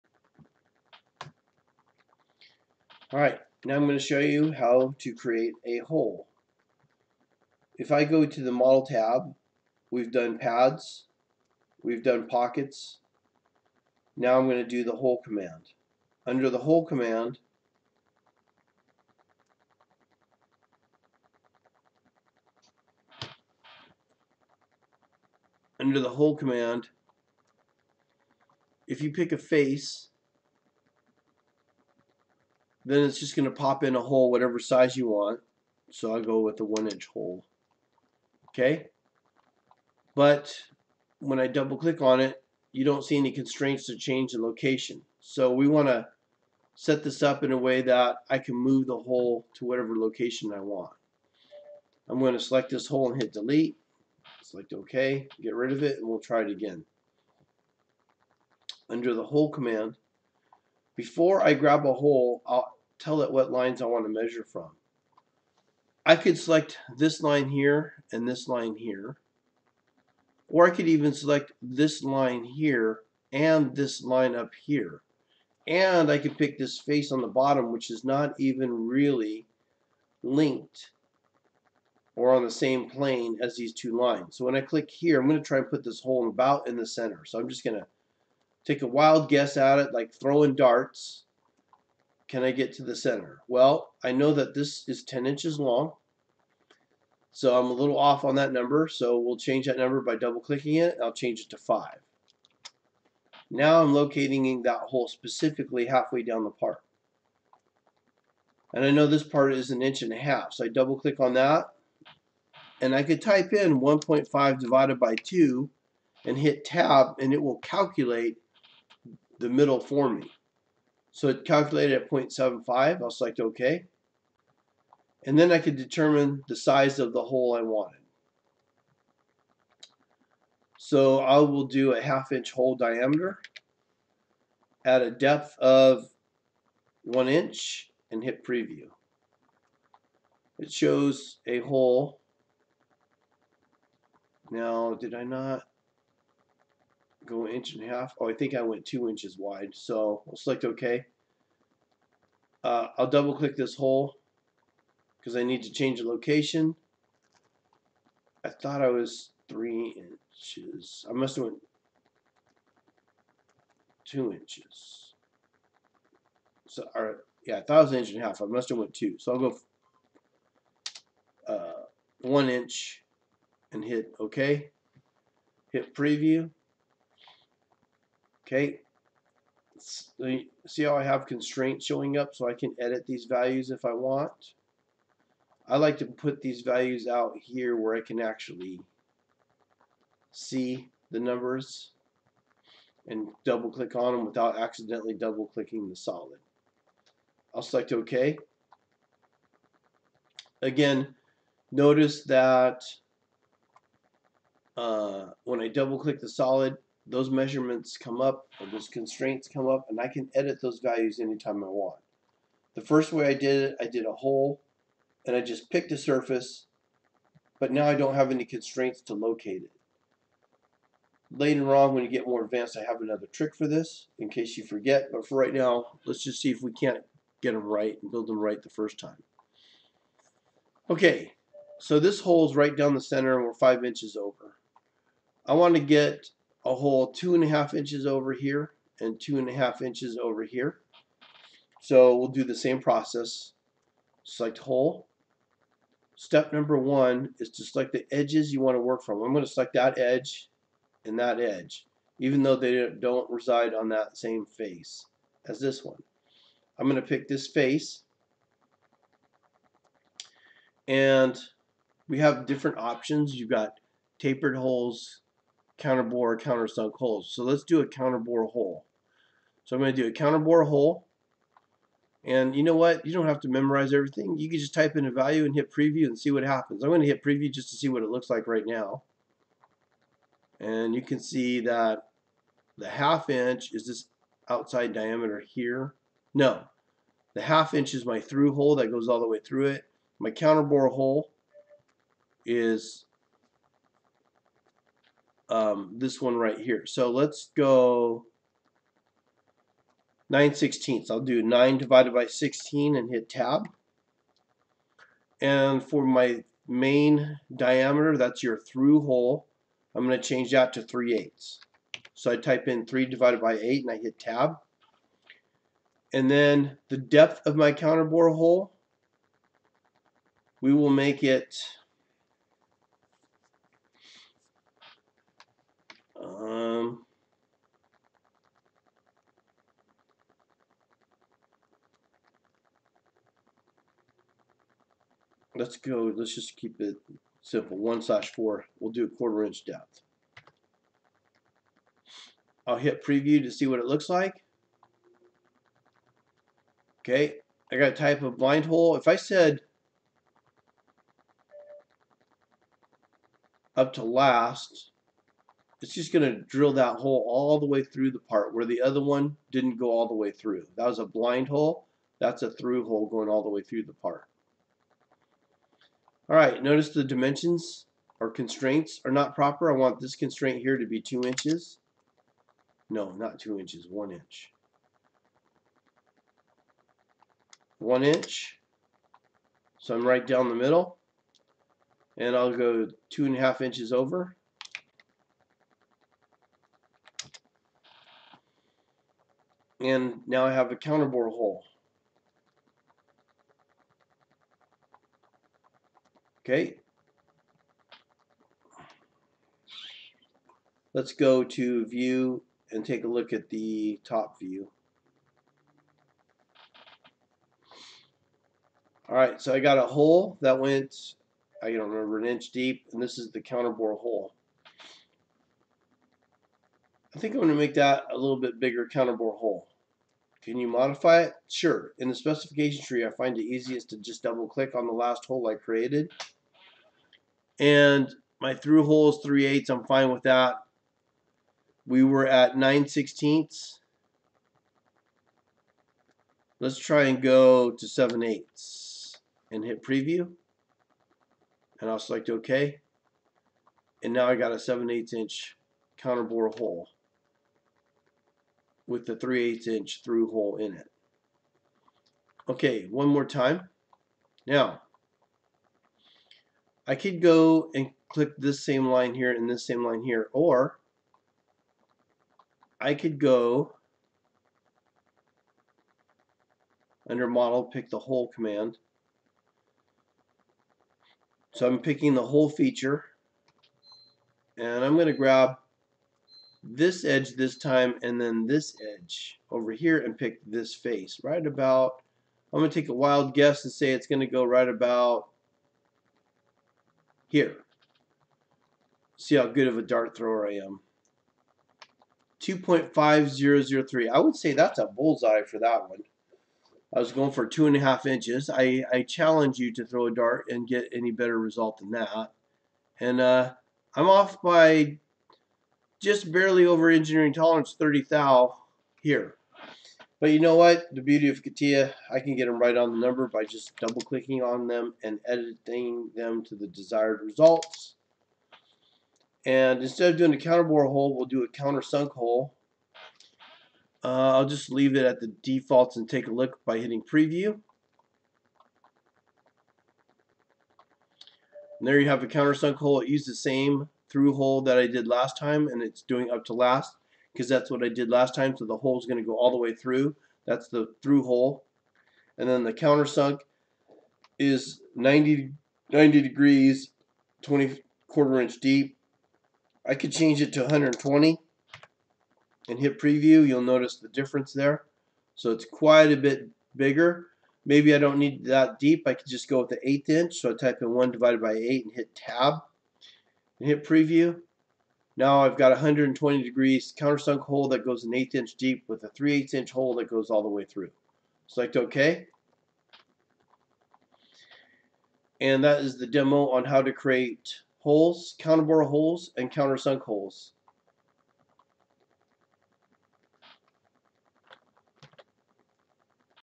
Counterbore Countersink Hole Feature CATIA 3D Experience
This video was created to help students currently enrolled at EvCC
Demo: Counterbore Countersink Hole Feature CATIA 3D Experience